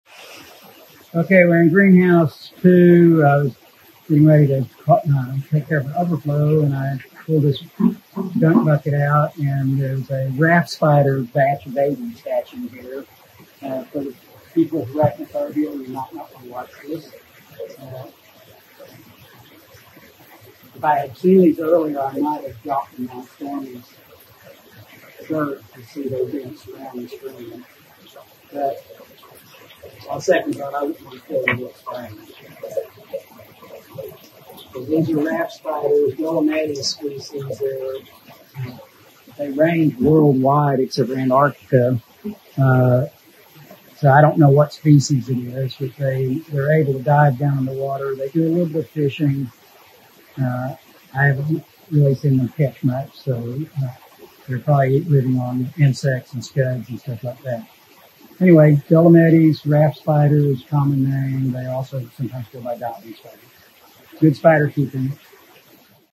BABY SPIDERS! - Tropical Fish Hatchery
Batch of baby Raft Spiders (Dolomedes species)